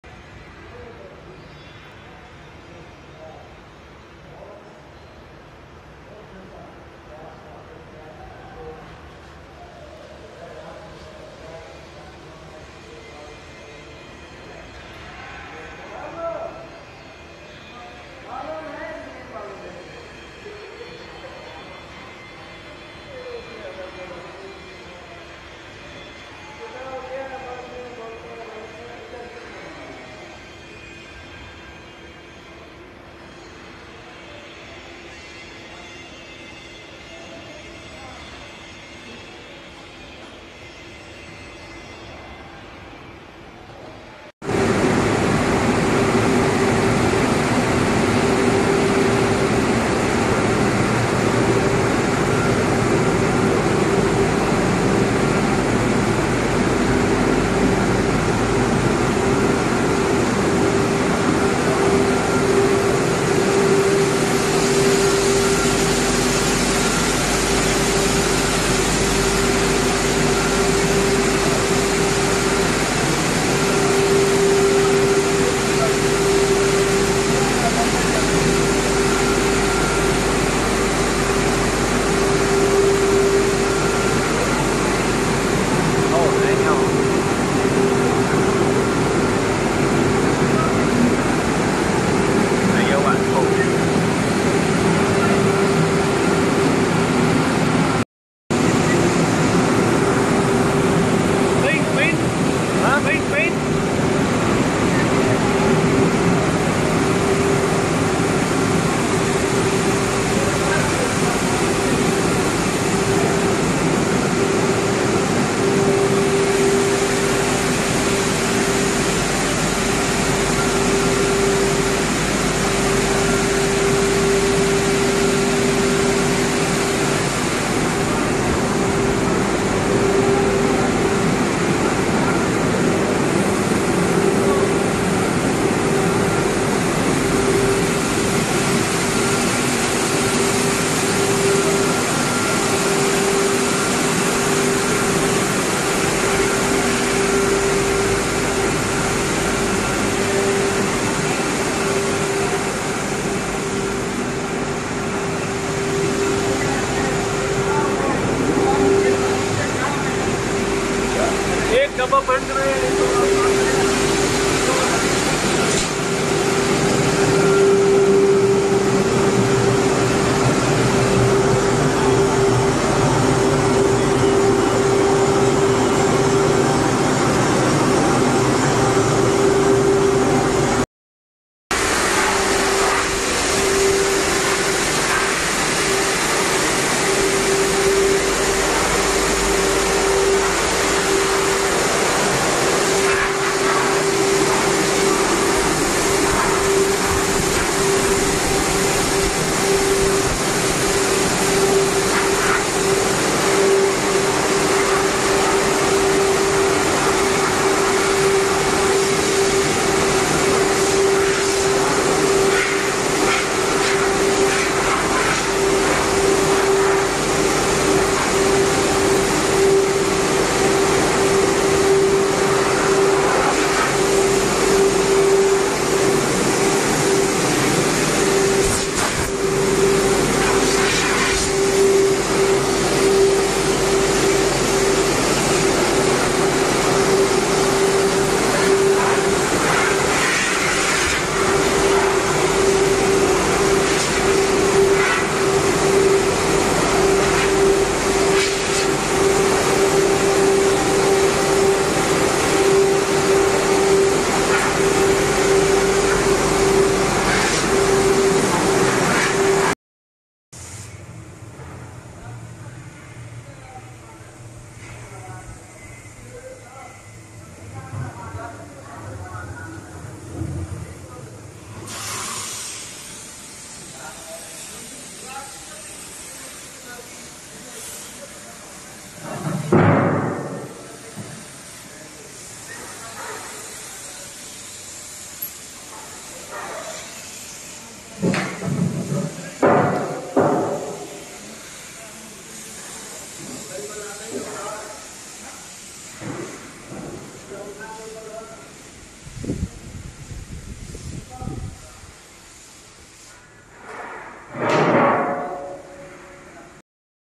QAT3730 Tunnle type shot blasting machine+Painting & Drying line
QAT3730 Tunnel type Shot blasting machine is a continuous catenary shotblast cleaning equipment, mainly by the room the shotblast cleaning, shot blasting machine assembly, pellet material circulation purification system, hanging chain conveyor, sealing and auxiliary chamber, the projectile collection system, workpiece rotation mechanism, dust removal system and control components.

Richard Lee
Sales Manager
Address:​No.867 Century Avenue, Huangdao district, Qingdao,China